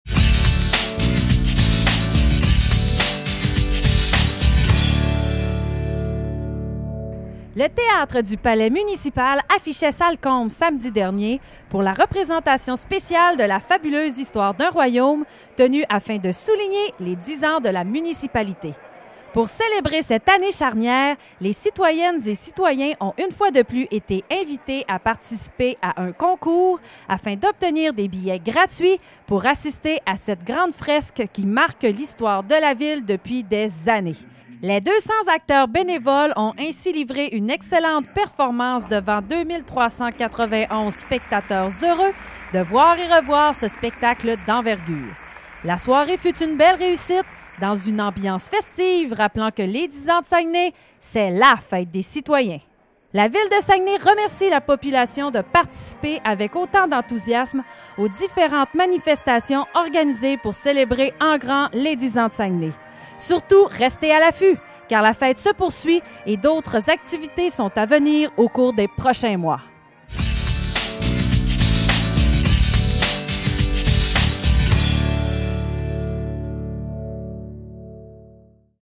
Salle comble à la Fabuleuse pour les 10 ans de Saguenay
Le Théâtre du Palais municipal affichait salle comble le samedi 7 juillet dernier pour la représentation spéciale de La Fabuleuse Histoire d'un Royaume tenue afin de souligner les 10 ans de la municipalité.La soirée fut une belle réussite, dans une ambiance festive rappelant que les 10 ans de Saguenay, c'est la fête des citoyens.